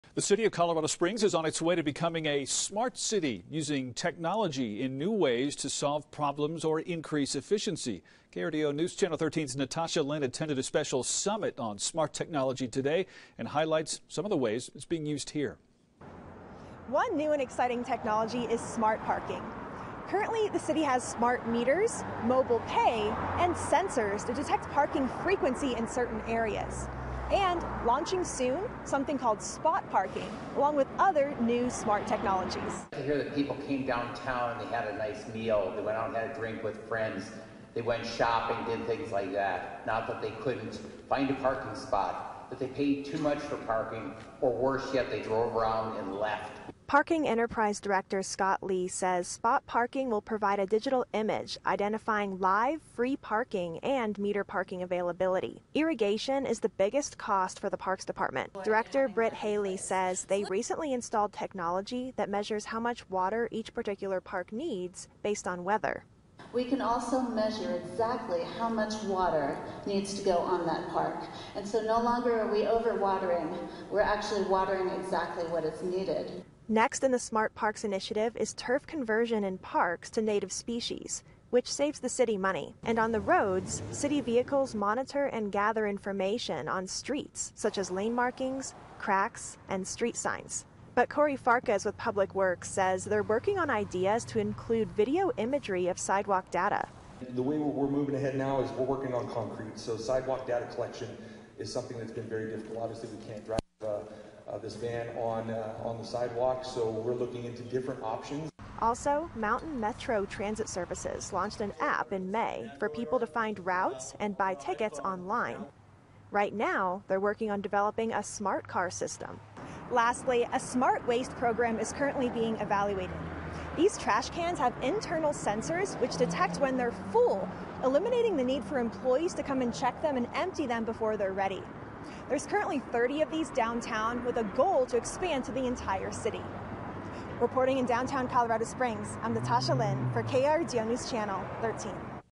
New technological solutions coming to Colorado Springs to become a "smart city
The City of Colorado Springs is on its way to becoming a "smart city." According to city officials, that means finding different ways new technology can help solve problems in the Pikes Peak Area.